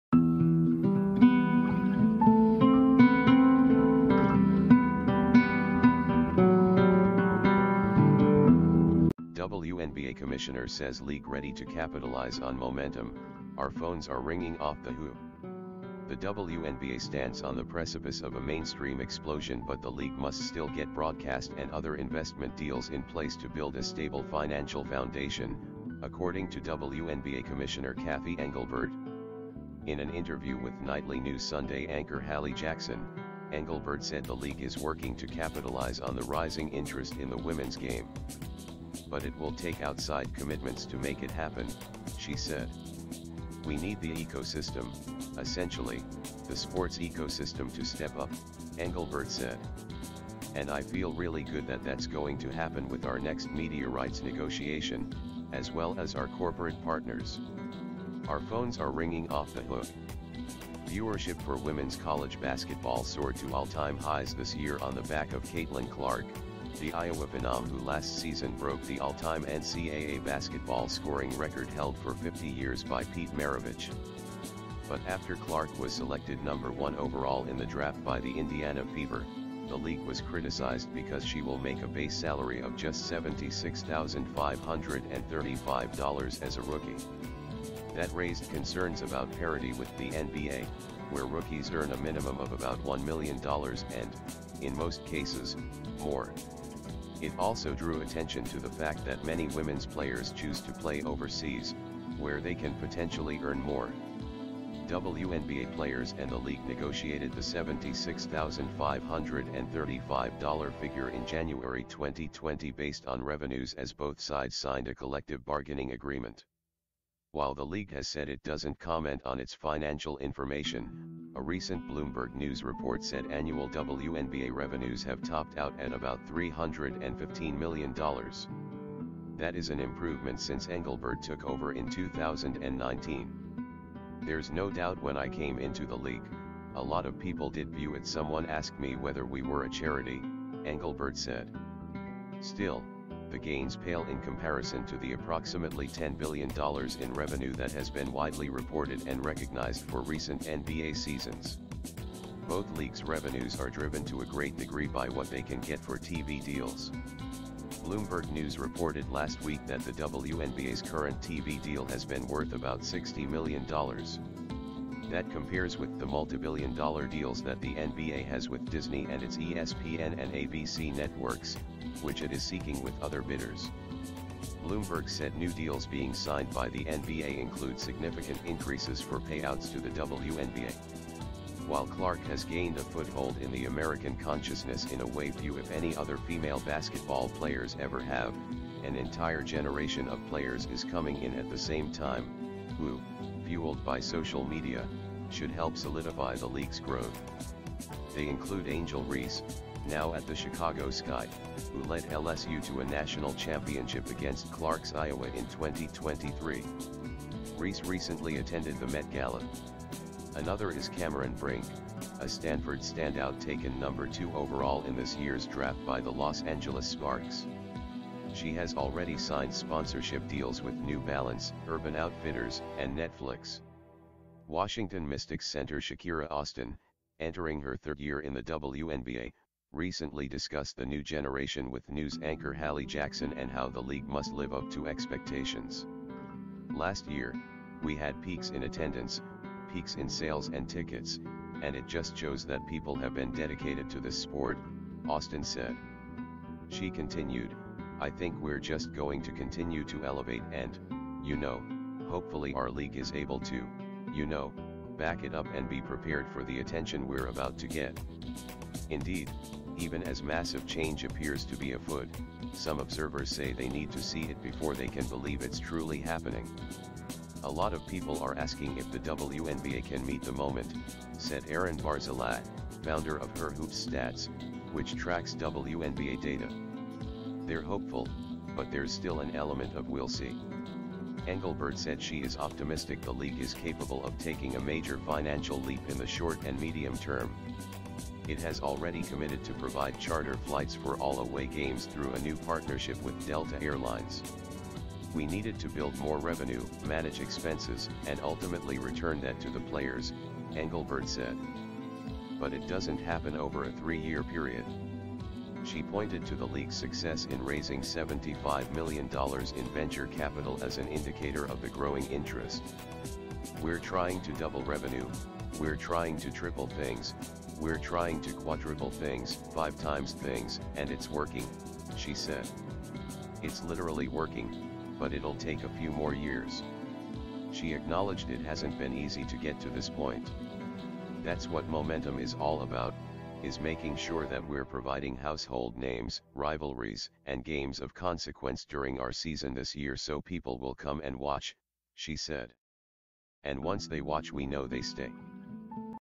WNBA commissioner says league ready to capitalize on momentum Our phones are ringing off the hoo👀
That raised concerns about parity with the NBA, where rookies earn a minimum of about $1 million and, in most cases, more. It also drew attention to the fact that many women's players choose to play overseas, where they can potentially earn more.

WNBA players and the league negotiated the $76,535 figure in January 2020 based on revenues as both sides signed a collective bargaining agreement. While the league has said it doesn’t comment on its financial information, a recent Bloomberg News report said annual WNBA revenues have topped out at about $315 million.

That is an improvement since Engelbert took over in 2019.

"There’s no doubt when I came into the league, a lot of people did view it — someone asked me whether we were a charity," Engelbert said.

Still, the gains pale in comparison to the approximately $10 billion in revenue that has been widely reported and recognized for recent NBA seasons.

Both leagues' revenues are driven to a great degree by what they can get for TV deals. Bloomberg News reported last week that the WNBA's current TV deal has been worth about $60 million. That compares with the multibillion-dollar deals that the NBA has with Disney and its ESPN and ABC networks, which it is seeking with other bidders.